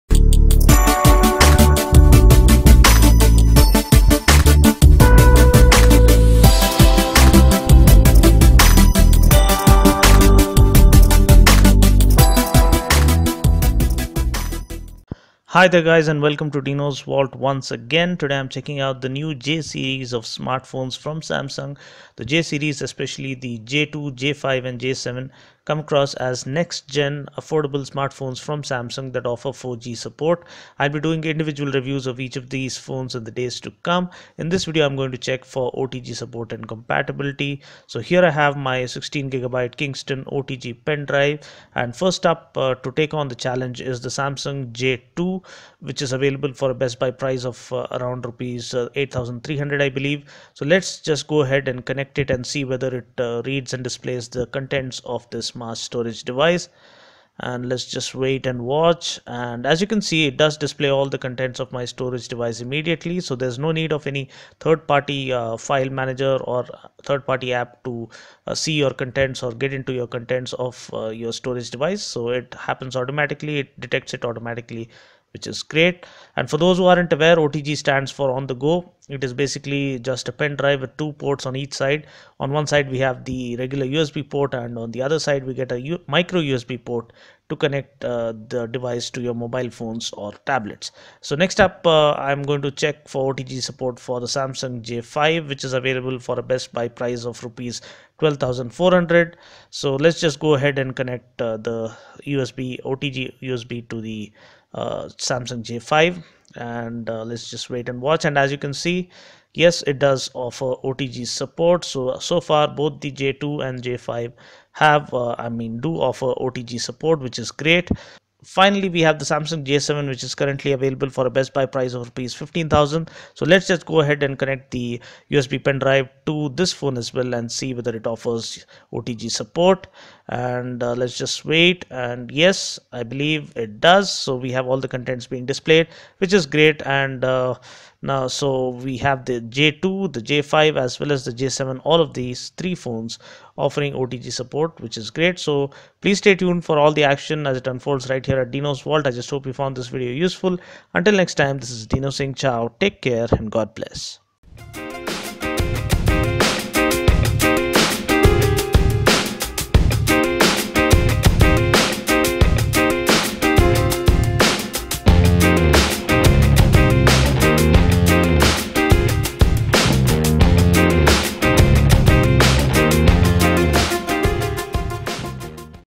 Mobiles@Dinos: Samsung Galaxy J2, J5, J7 OTG Support/Compatibility Test
In this video, I test for OTG support/compatibility for Samsung Galaxy J2, J5, and J7 4G smartphones.